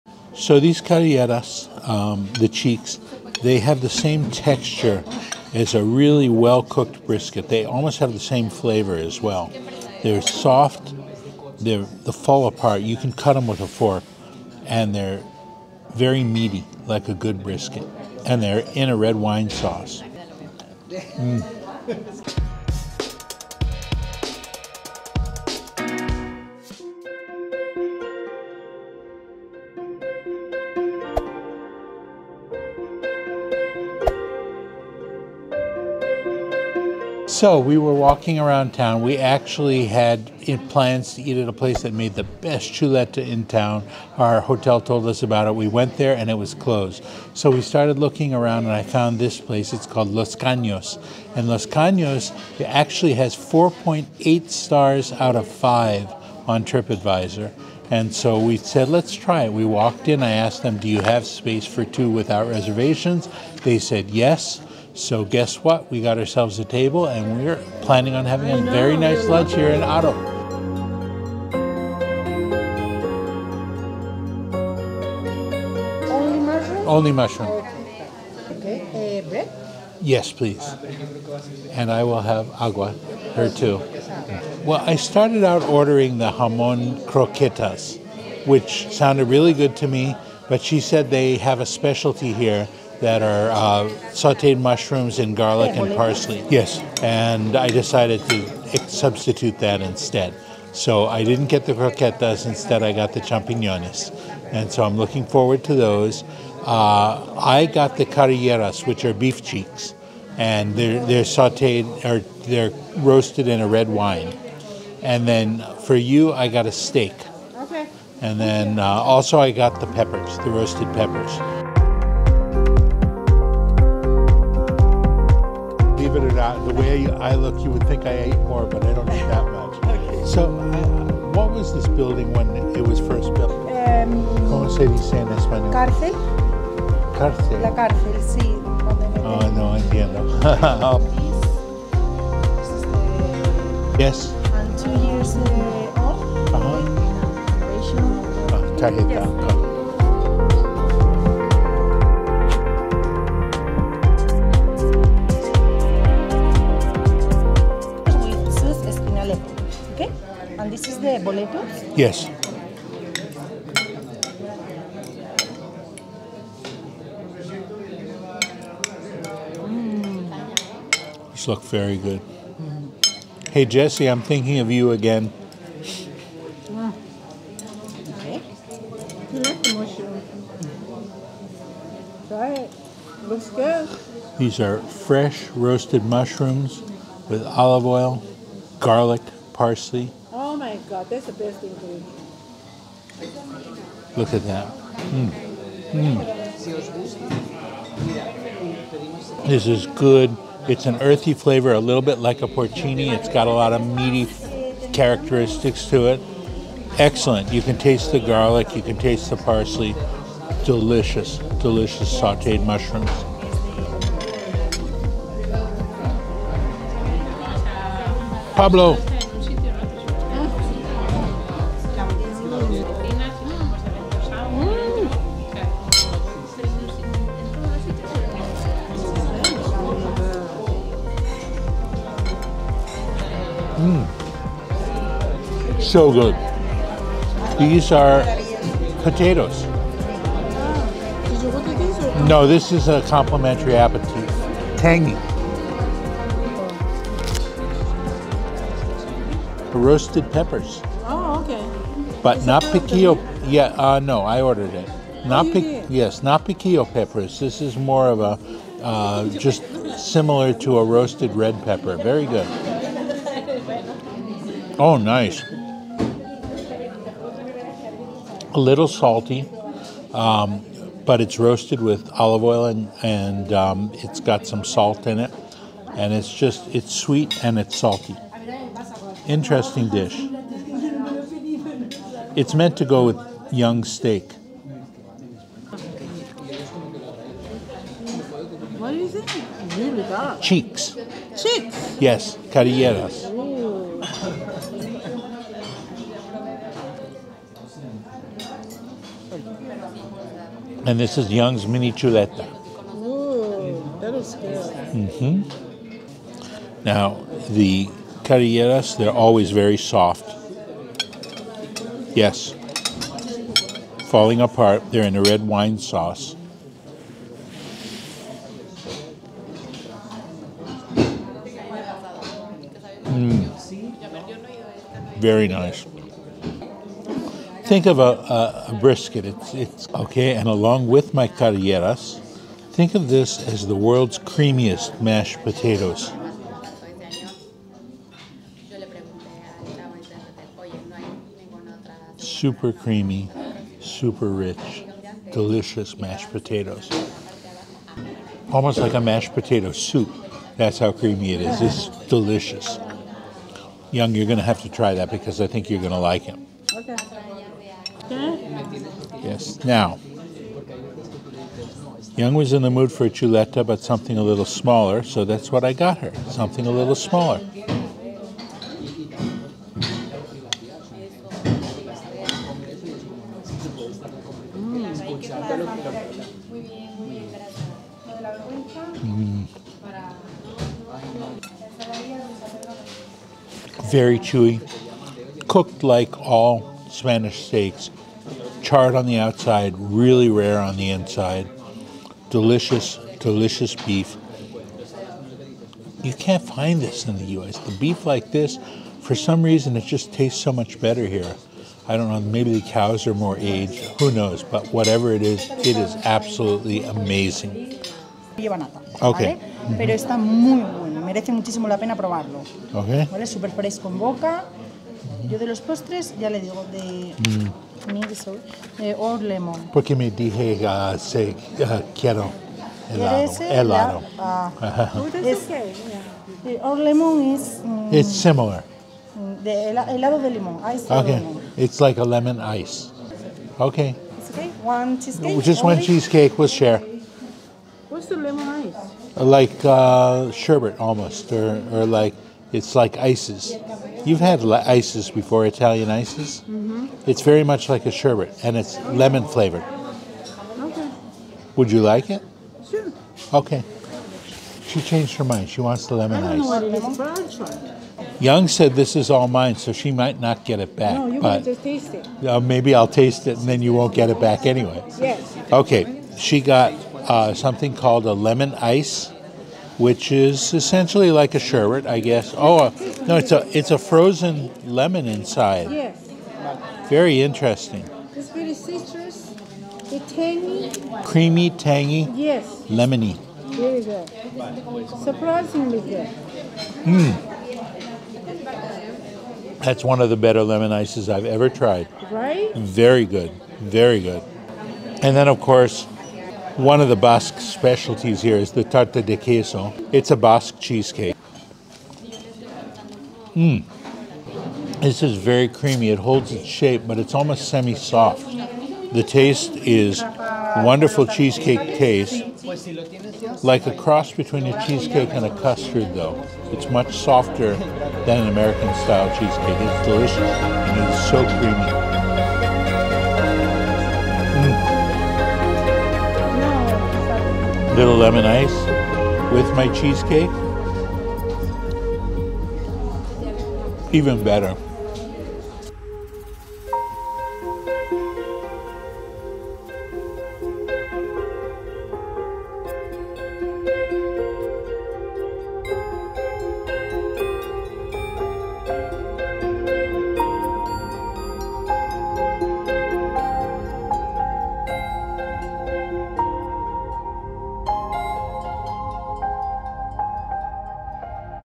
Eating LIKE A LOCAL in Rioja Spain's HISTORIC Buildings!
Join us on a culinary adventure as we explore the rich flavors of traditional Spanish food in the stunning region of Rioja, Spain! In this video, we step inside a historic building that beautifully captures the essence of Spanish culture and cuisine. From mouth-watering tapas to exquisite local wines, discover the authentic tastes that make Rioja a gastronomic paradise.

Experience the vibrant atmosphere as we delve into the history of the building and its significance in the heart of Rioja. Watch as we interact with locals and food enthusiasts, sharing their passion and expertise in creating traditional dishes that have been passed down through generations.

Whether you're a foodie, a travel enthusiast, or simply curious about Spanish culture, this video is for you!